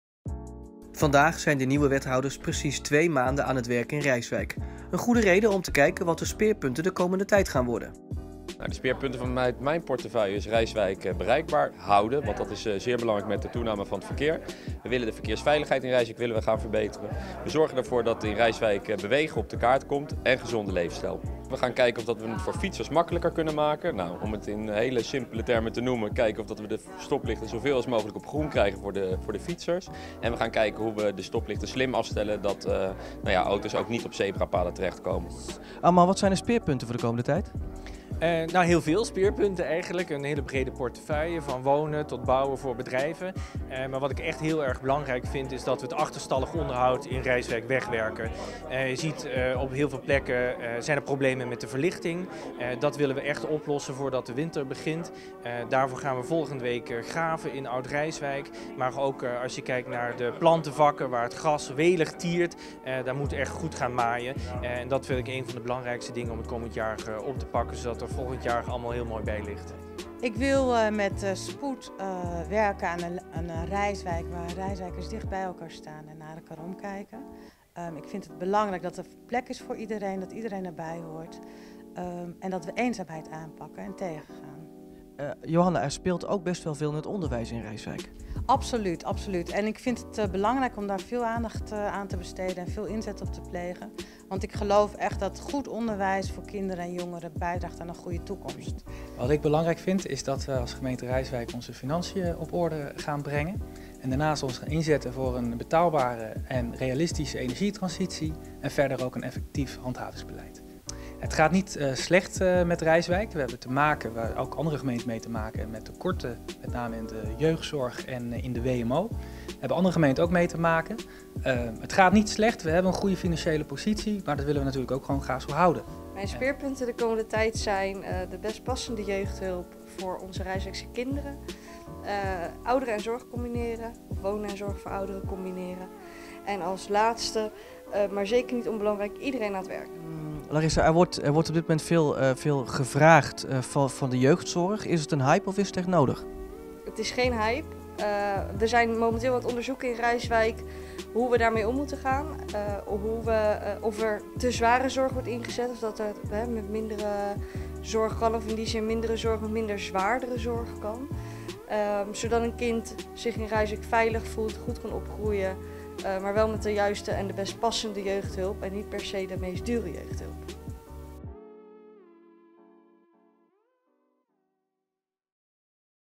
Speerpunten college
Welke speerpunten hebben de wethouders de komende tijd?  Larissa Bentvelzen, Armand Van de Laar, Björn Duncan Lugthart, Johanna Besteman en Jeffrey Keus lieten het weten tijdens een persbijeenkomst in het stadhuis.